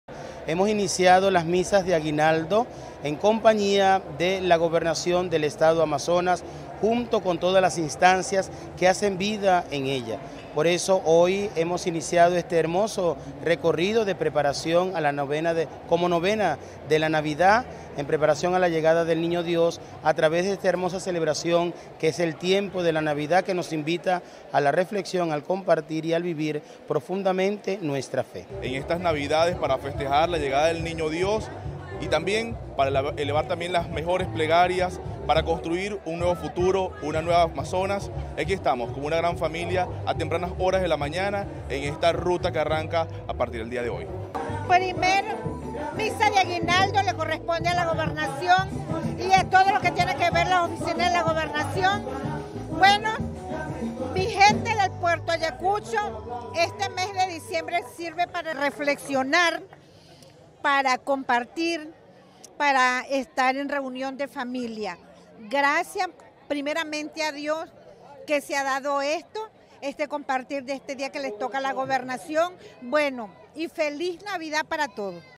ENVIDEO || INICIAN LAS MISAS DE AGUINALDOS EN AMAZONAS
Amazonas se prepara para la llegada del Niño Dios a través del inicio de la tradicional Novena de la Navidad con la 1era Misa de Aguinaldo celebrada por la Gobernación del Estado.